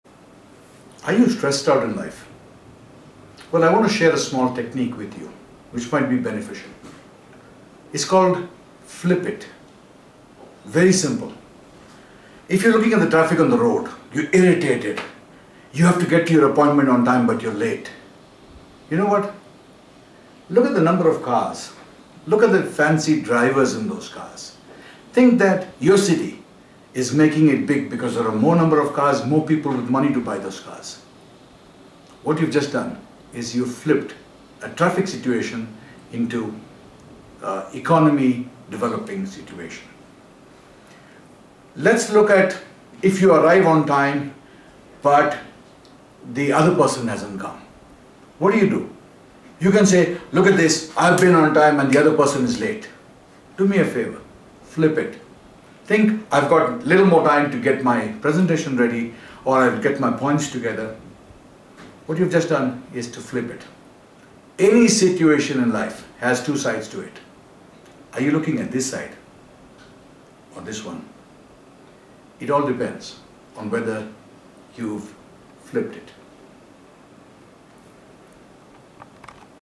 Life Challenges us - Flip it !
The best way to build your positivity is to get the mind to work how you want it to. Here is a powerful technique that will get you into a new, happy zone.